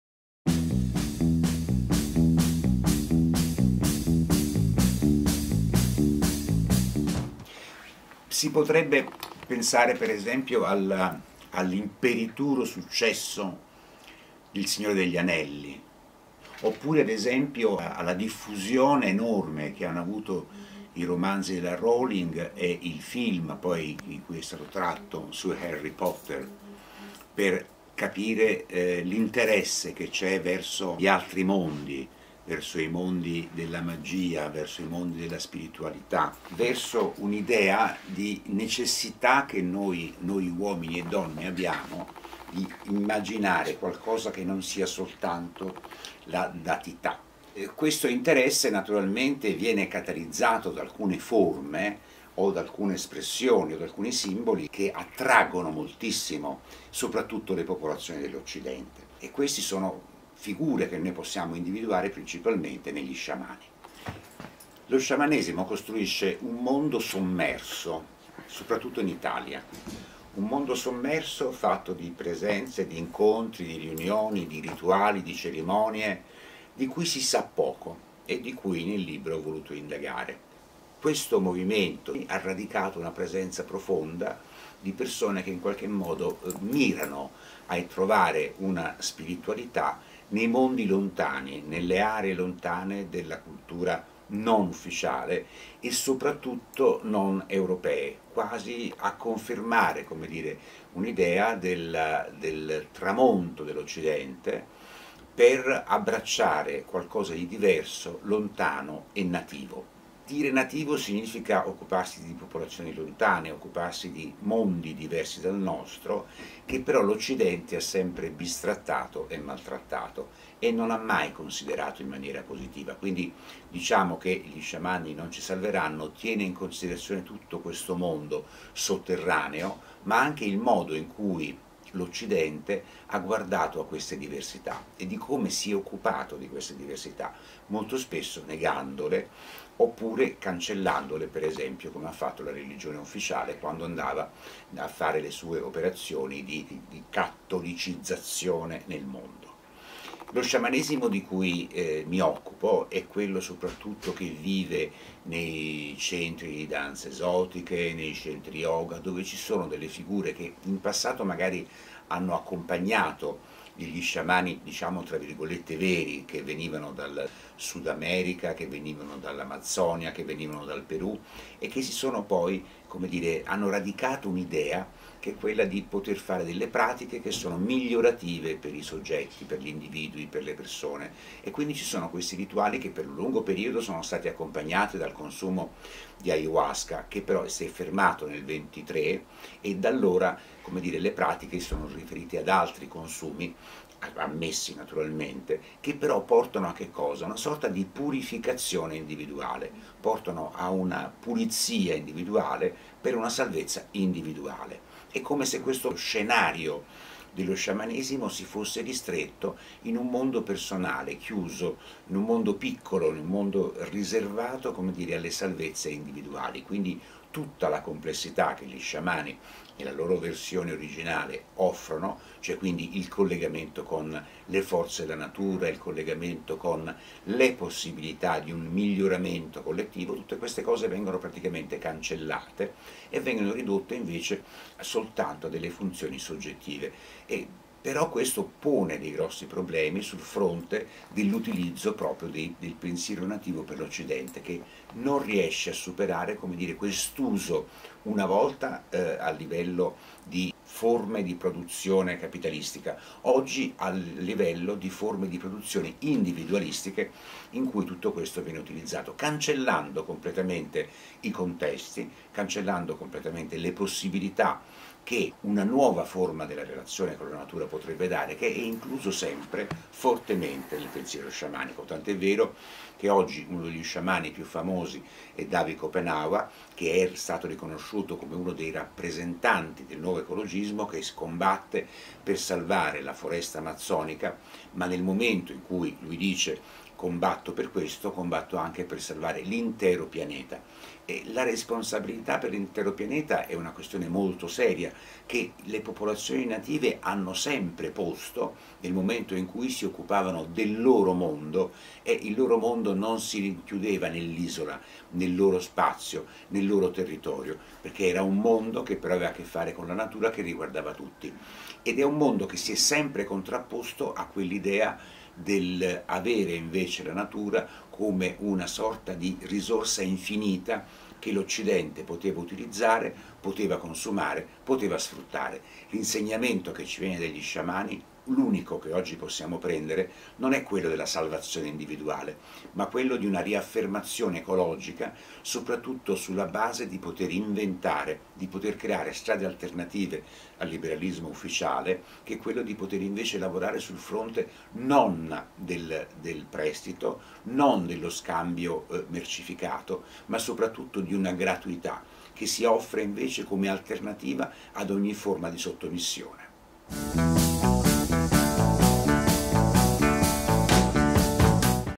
ON THE COUCH - STEFANO DE MATTEIS - GLI SCIAMANI NON CI SALVERANNO
Stefano de Matteis ci presenta il suo ultimo libro "Gli sciamani non ci salveranno"

Le nuove spiritualità offrono l'arcaico, l'originario e il mondo ancestrale come un parco giochi dove ritrovare la propria intimità per una rinascita individuale. Ma il pensiero nativo non è affatto addomesticato e può insegnarci regole fondamentali come il farsi carico e il prendersi cura. I benefici che la natura ci offre vanno restituiti: non si può solo prendere. In questo è inclusa un'importante regola morale: non è concessa la libertà di arricchirsi a spese della natura, umana e non.

Gli atei di oggi sono molto credenti: più l'Occidente tramonta, più il materialismo dei consumi e della tecnologia diventa irrinunciabile e più si cerca riparo in spiritualità alternative con cocktail autoprodotti di alchimia e tarocchi, metempsicosi, ufo ed esperienze extracorporee... Ma tra lo yoga e la meditazione uno spazio consistente lo ritaglia un nugolo di sedicenti sciamani che invadono la scena sotterranea del misticismo e guidano una vasta clientela alla ricerca di sé. Alle loro spalle si agitano fantasmi molto reali: dal falso sciamano di Capitol Hill a Davi Kopenawa, vero sciamano yanomami divenuto nuova stella dell'ecologia, da Black Elk, Lakota in odore di santità, fino a un misconosciuto saggio maori, Tamati Ranapiri, che ha lasciato una lezione inascoltata dall'Occidente. A partire da qui e al di là dello sciamanesimo «new age», il pensiero nativo, per lo più ignorato quando non dileggiato, andrebbe preso sul serio per le indicazioni importanti che ci offre per il nostro futuro, a cominciare dalla responsabilità di prenderci cura della natura di cui facciamo parte, dopo averla per secoli solo sfruttata.

Stefano De Matteis, antropologo, insegna all'Università degli Studi Roma tre ed è professore invitato alla Pontificia Università Gregoriana. Si è a lungo occupato di rappresentazioni simboliche, pratiche performative e processi rituali diffondendo in Italia l'antropologia della performance e l'antropologia dell'esperienza. Ha curato la pubblicazione delle opere di Ernesto de Martino e l'edizione italiana delle opere di Victor Turner. Tra le sue ultime ricerche: Le false libertà. Verso la postglobalizzazione (2017) e Il dilemma dell'aragosta, la forza della vulnerabilità (2021). Collabora stabilmente con Radio Rai 3 e scrive per il domenicale del "Sole 24 ore".